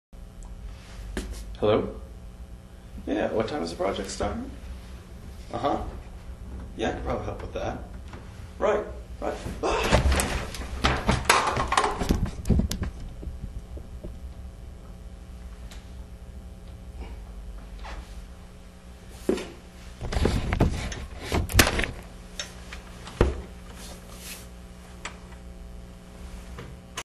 IUB Furniture Open House Commercial #2
Commercial to promote the Indiana University Bloomington Furniture Open House.  The Open House is hosted by the Purchasing Department and will be held Wednesday, October 28, 2009, from noon to 3 p.m. in the Poplars Building, 4th Floor.